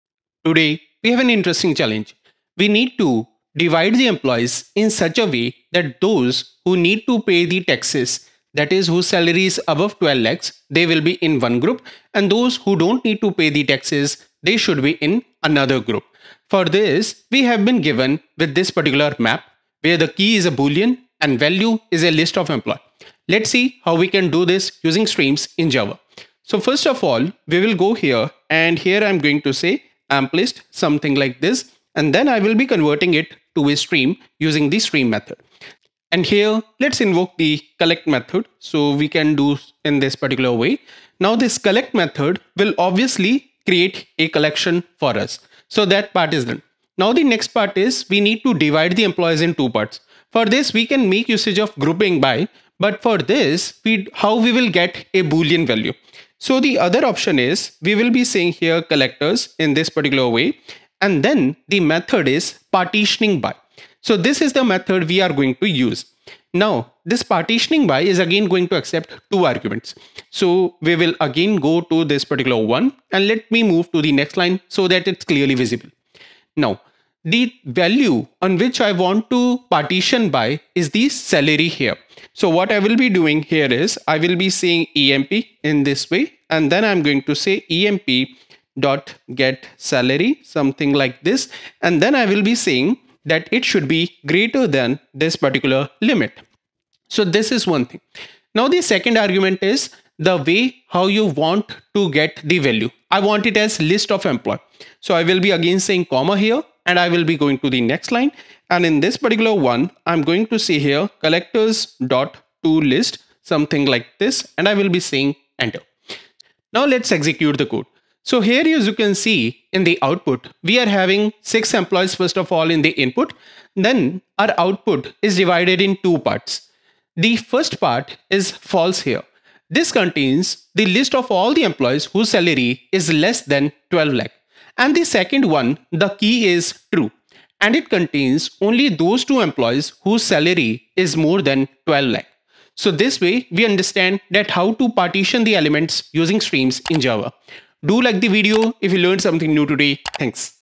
Day-53: Partitioning elements using Streams in Java
In this short we will see how to partition elements using Stream in Java.